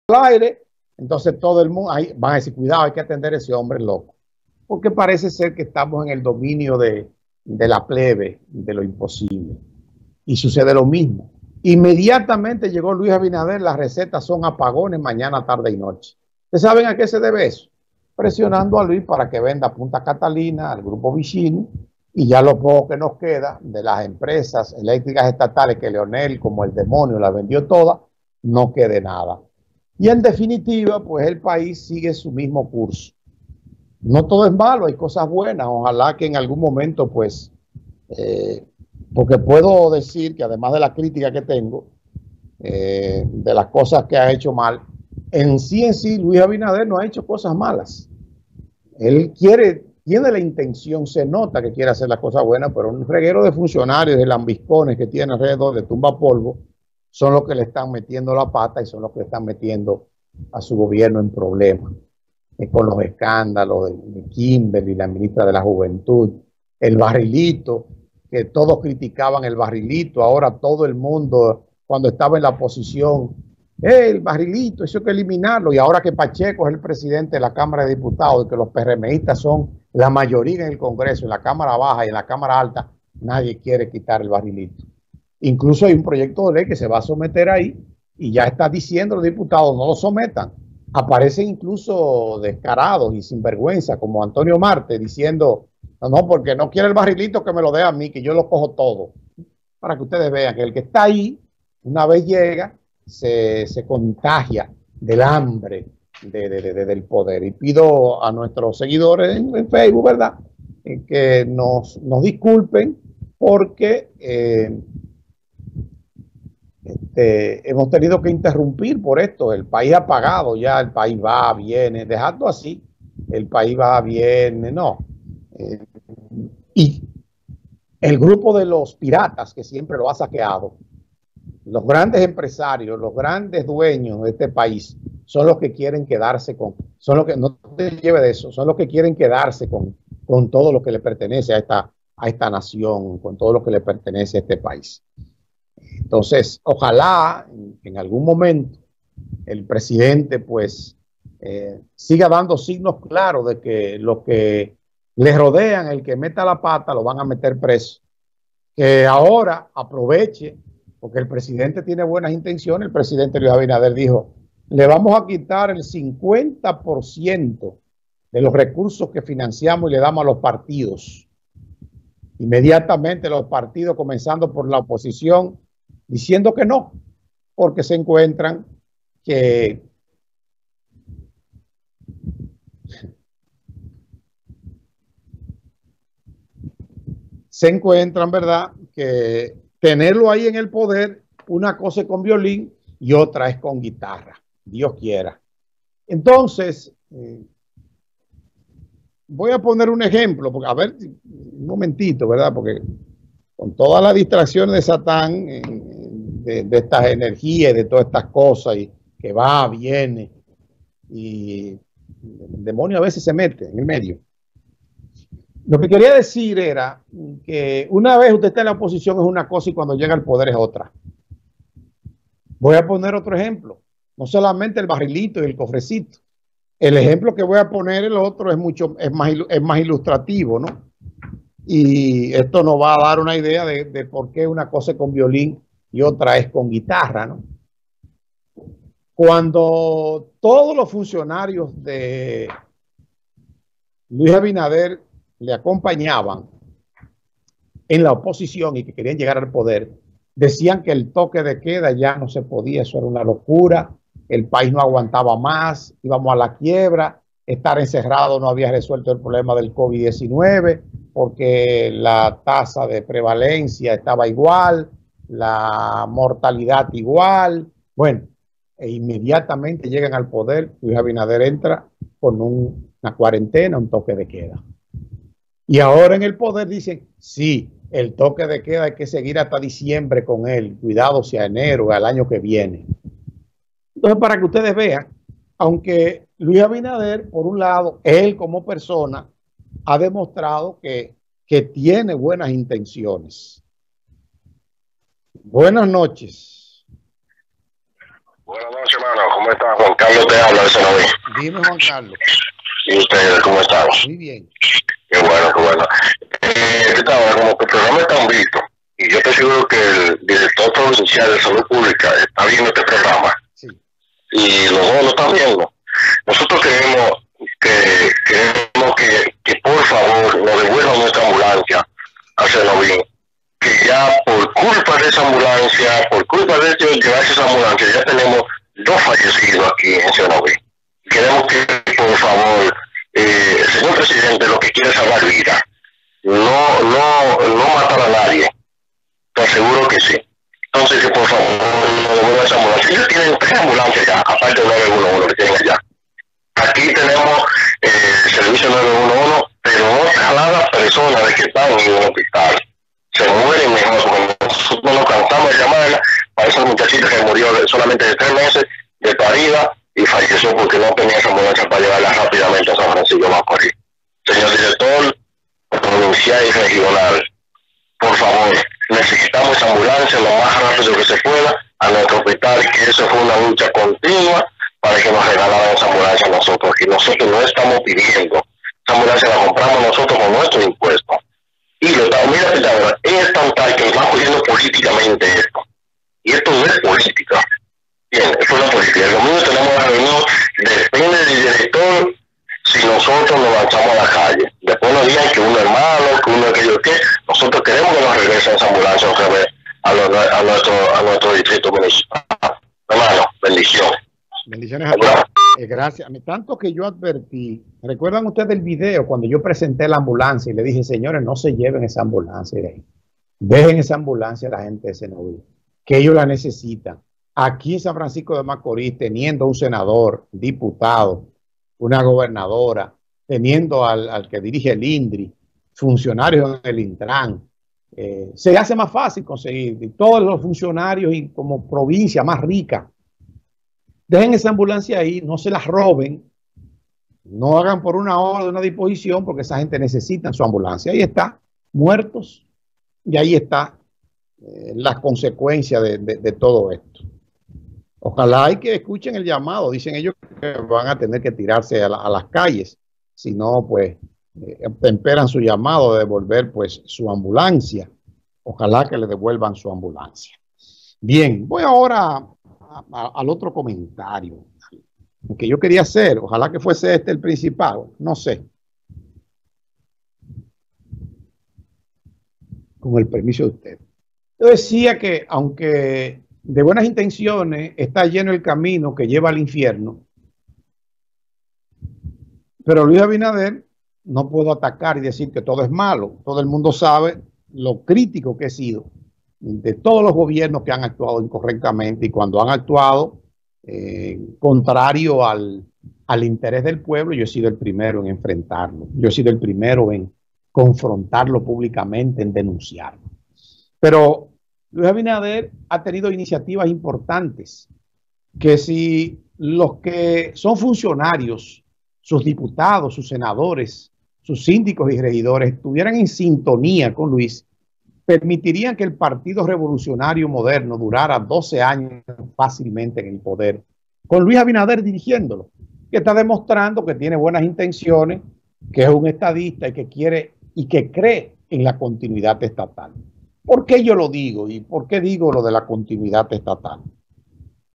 Luis Abinader y su gobierno se dan cuenta que “una cosa es con violín y otra con guitarra”
Luis Abinader y su gobierno se dan cuenta que “una cosa es con violín y otra con guitarra” Dice Pitágoras Vargas